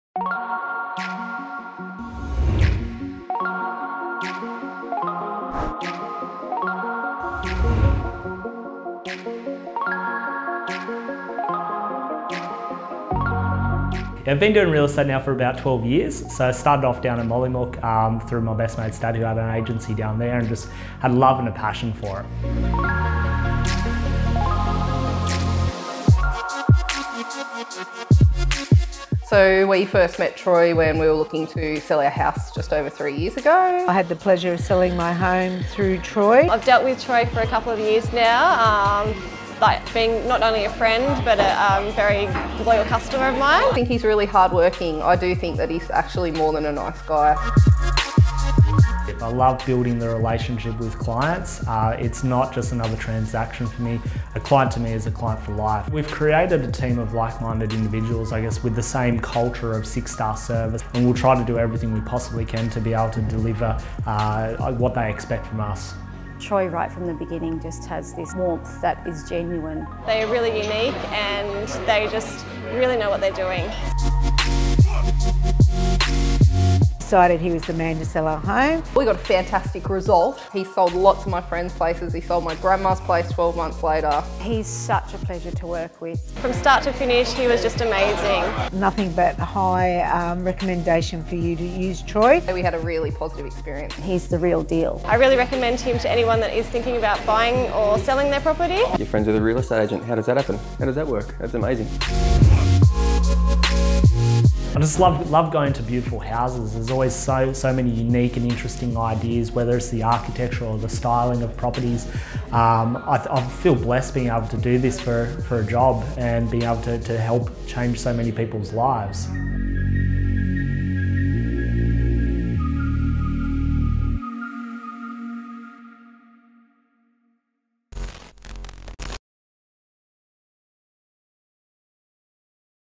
Troy McNeice | Profile Video | molenaar + mcneice
passionate | approachable | determined | caring

Troy McNeice is a name familiar to most Northern Illawarra residents. With a passion for helping people, rock solid ethics and a contagious positivity, it’s no surprise that over 80% of Troy’s business is referrals or repeat clients.

Troy's determination, friendly approach and thorough understanding of the market has seen him achieve countless record sale prices and several industry awards over his ten years in the industry.

With an impressive track record of securing top prices for his clients ranging from apartments to prestigious million dollar residences. His vision for a better kind of agency has inspired him to open Molenaar + McNeice.